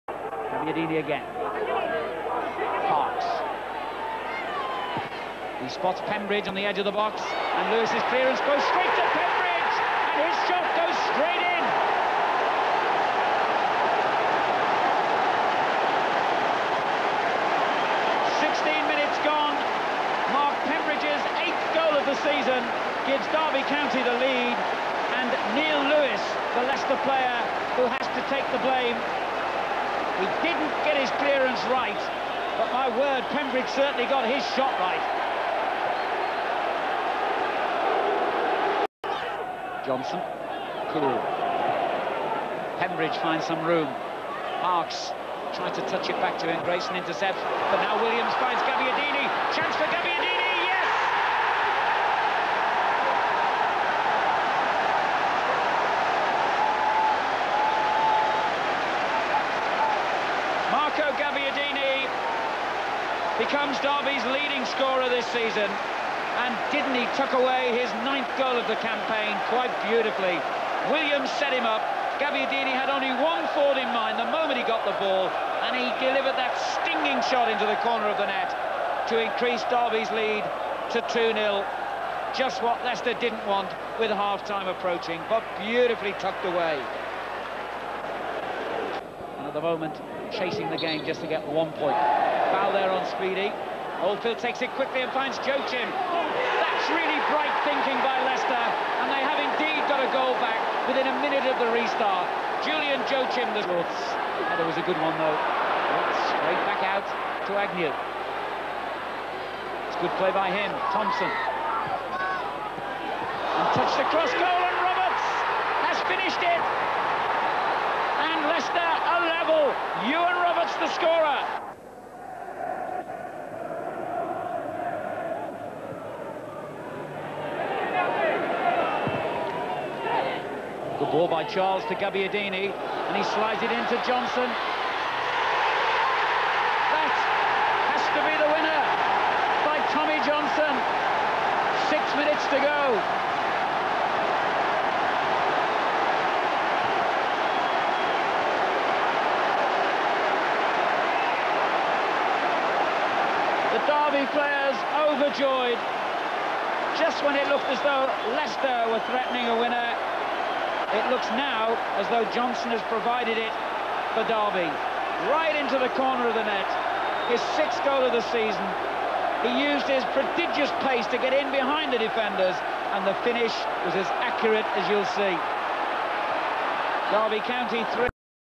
1993-94 Derby County 3 Leicester City 2 - 28/12/1993
Derby goals Mark Pembridge , Marco Gabbiadini , Tommy Johnson
Leicester goals Julian Joachim , Iwan Roberts